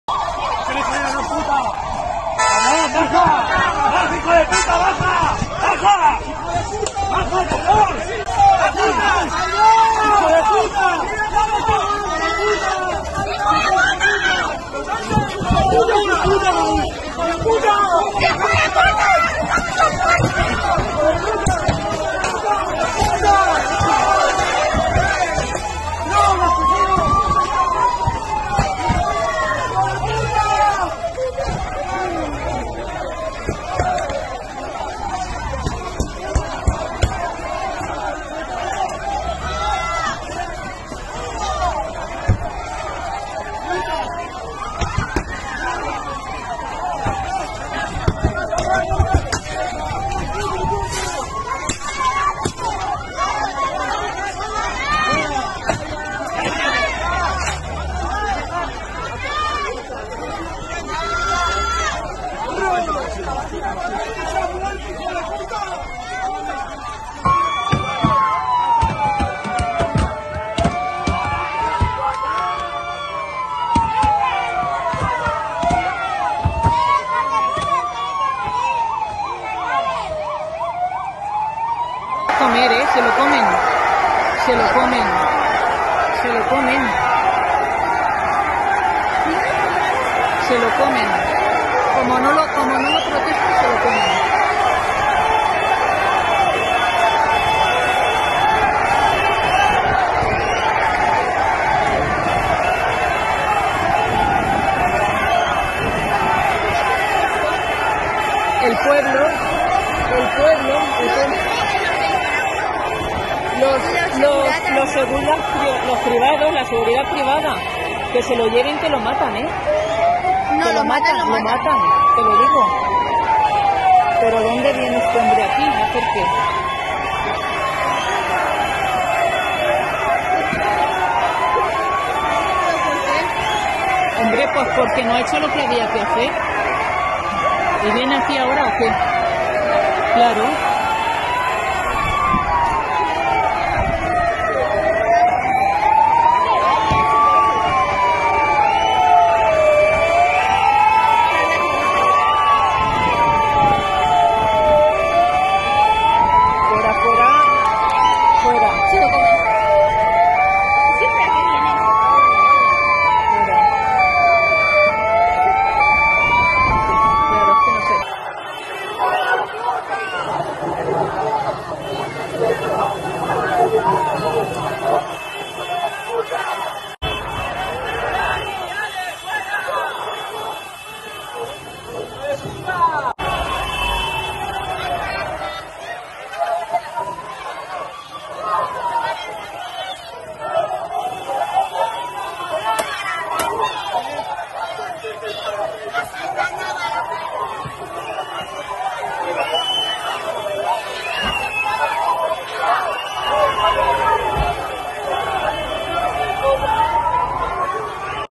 Así fue el DANTESCO PASEÍLLO DE SÁNCHEZ en Paiporta: "¡O se lo llevan o lo matan!"
Recapitulamos con distintas imágenes el paso a paso de los pocos metros que Pedro Sánchez caminó sobre el barro de Paiporta. Un reportaje de Bertrand Ndongo.